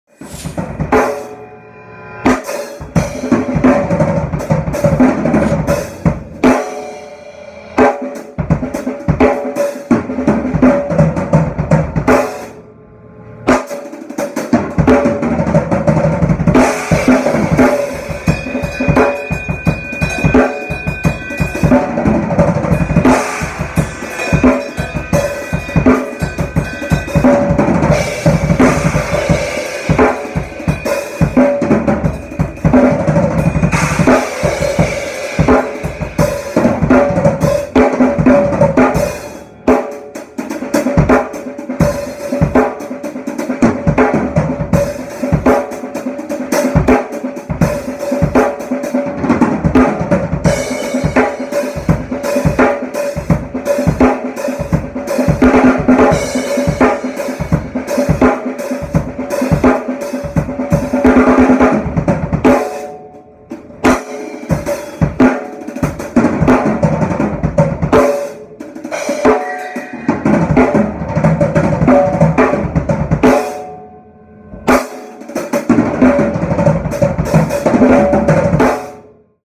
Michael Orris - Choppin’ Around 3
Choppin' around on a Monday afternoon. I like to go "choppin'".. Nice to have a room to play some drumssssss!